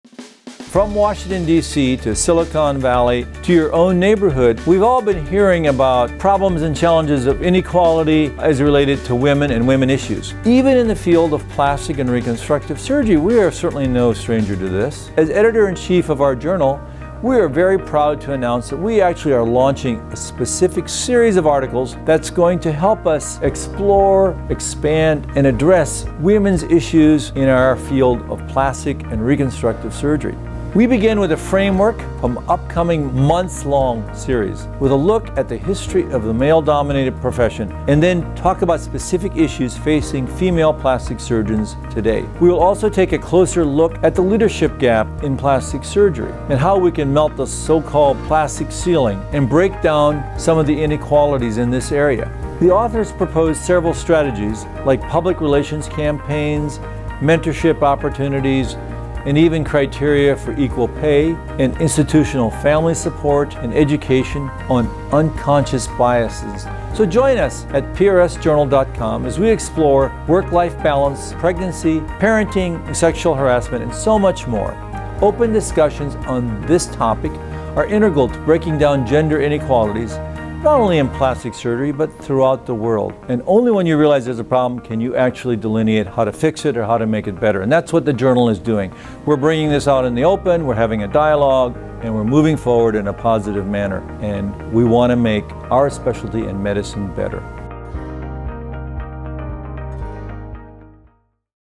"Women in Plastic Surgery" Special Topic Series– Plastic Surgery Hot Topics with Rod J. Rohrich, MD
In this video, Rod J. Rohrich, MD, Editor-in-Chief of “Plastic and Reconstructive Surgery” introduces the PRS "Women in Plastic Surgery Series." Be sure to keep an eye out for these articles over the next couple on months. Join the discussion on Twitter using ILookLikeAPlasticSurgeon.

The first Women in P.S. Series article appears in the September 2016 issue of Plastic and Reconstructive Surgery, Volume 138 Issue 3.

Read “Melting the Plastic Ceiling: Overcoming Obstacles to Foster Leadership in Women Plastic Surgeons” by Silva et al: